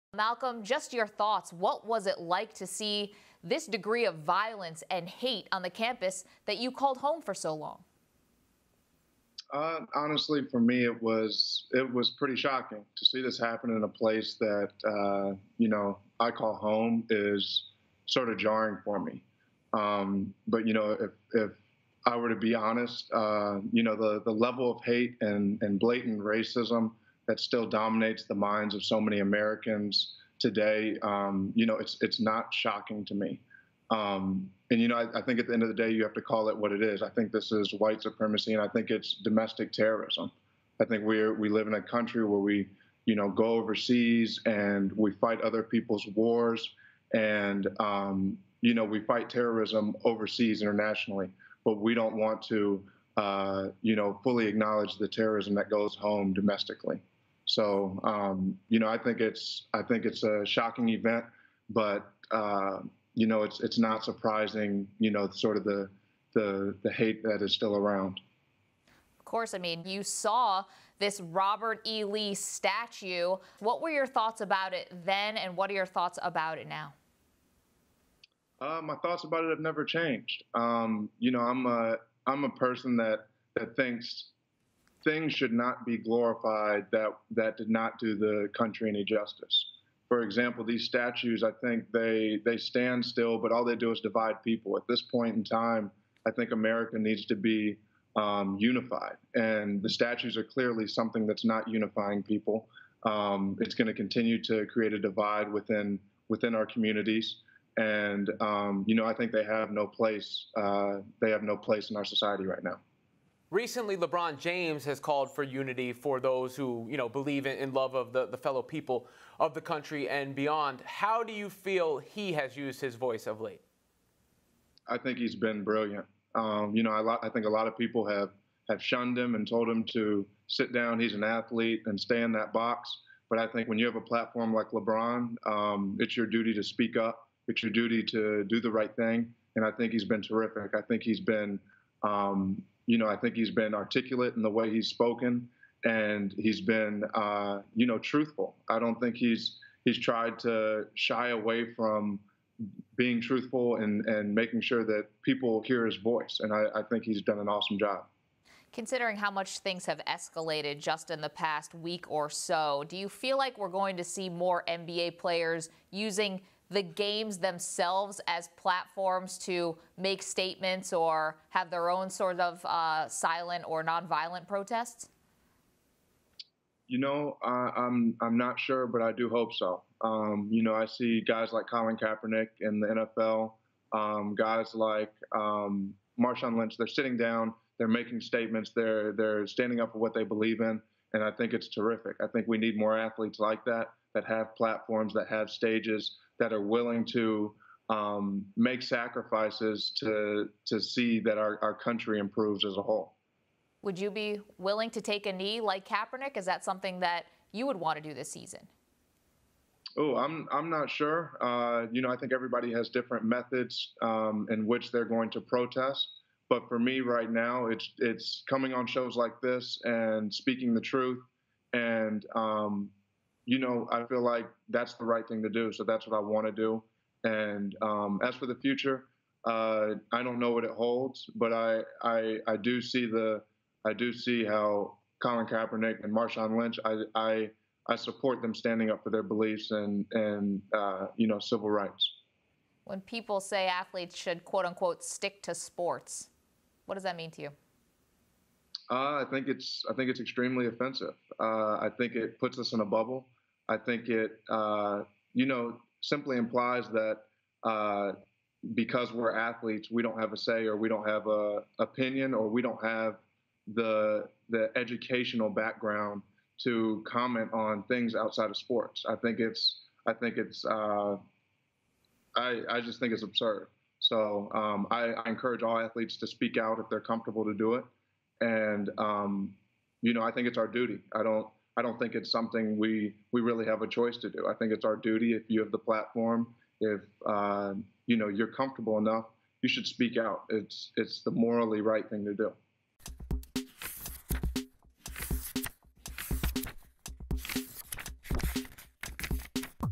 Ex-UVA Guard Malcolm Brogdon: 'Stick To Sports' Is Extremely Offensive | SI NOW | Sports Illustrated
In wake of the violence in Charlottesville, Milwaukee Bucks guard and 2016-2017 NBA Rookie of the Year Malcolm Brogdon gave a wide-ranging interview to SI Now about Confederate statues in America, the 'stick to sports' sentiment and national anthem protests.Subscribe